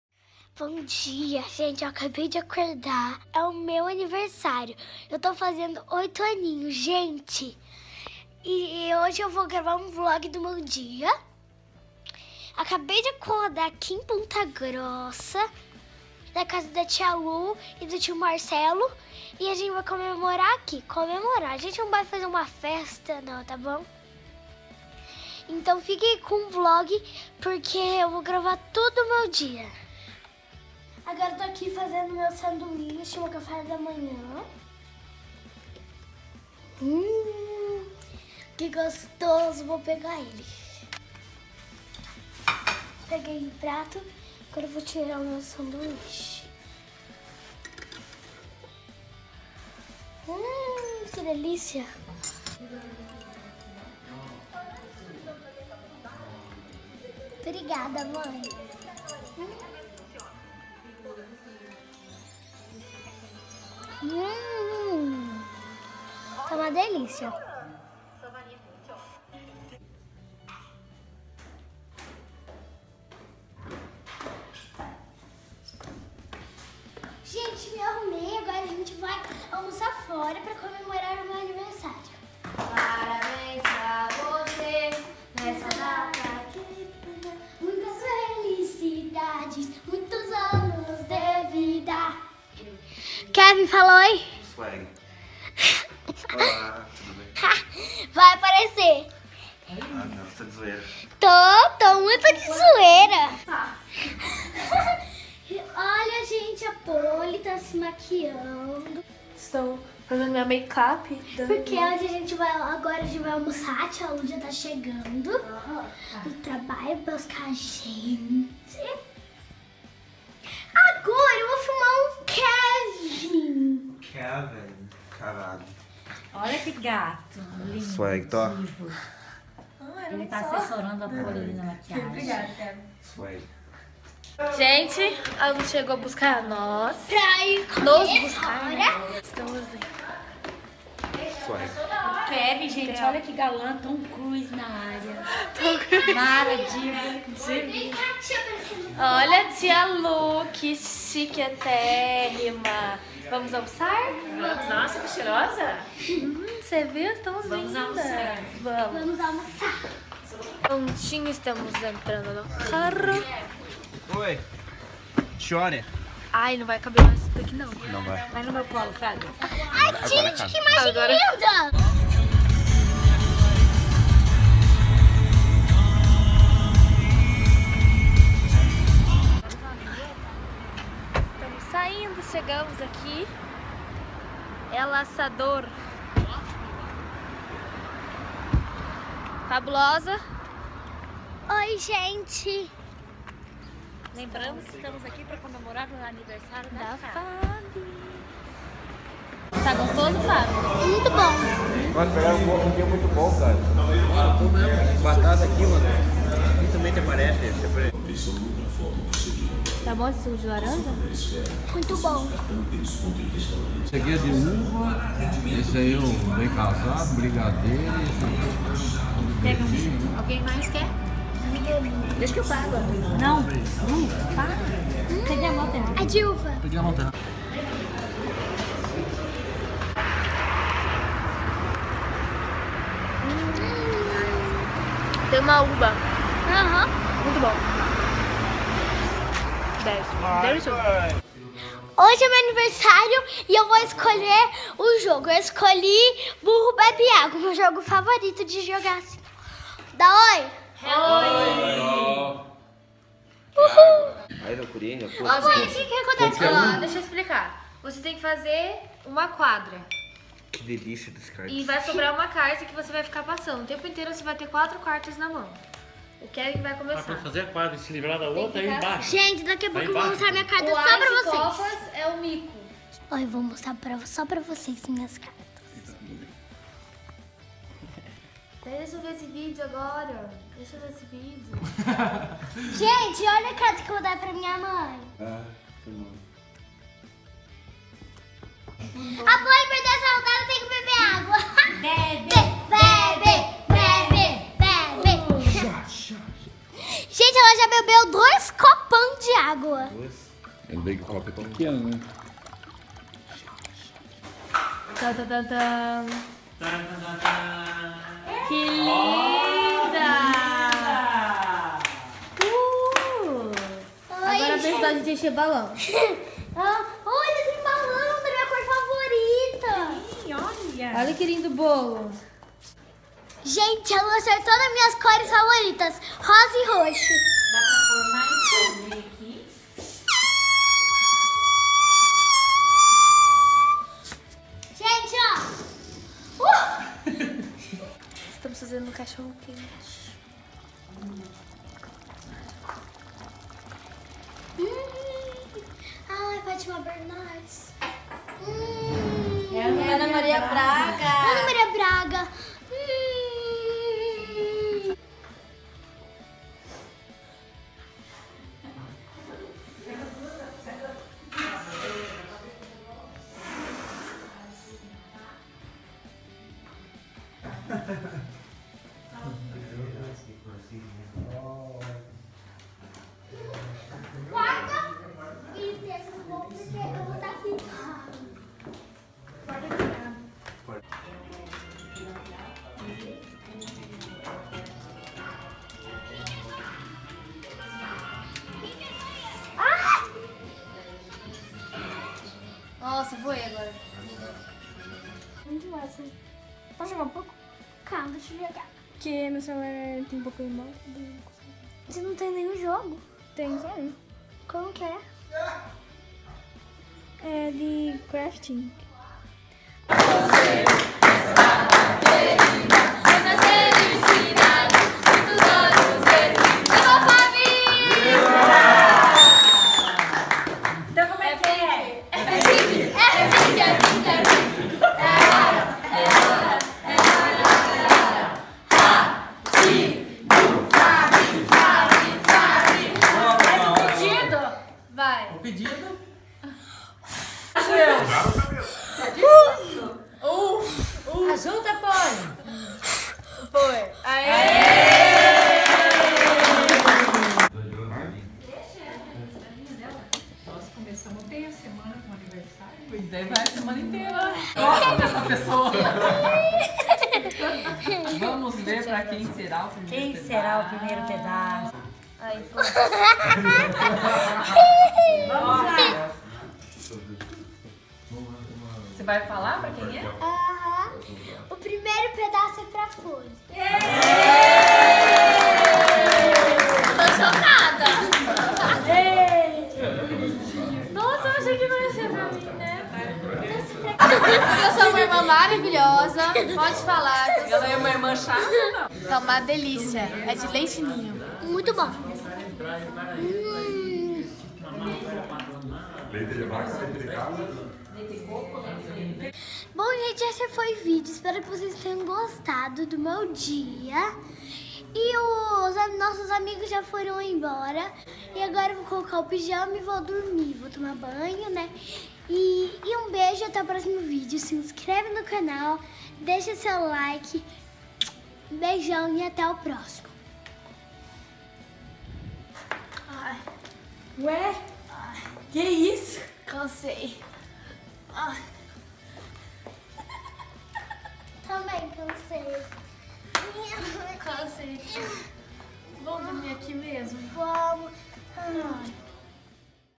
VIAGEM A PONTA GROSSA- ANIVERSÁRIO DA FABI
Passamos o meu níver na casa da tia Lu, foi muito legal ♥

musical.ly: @fabianalandim_

Endereço para correspondencias ou presentes:
Landimas
79008970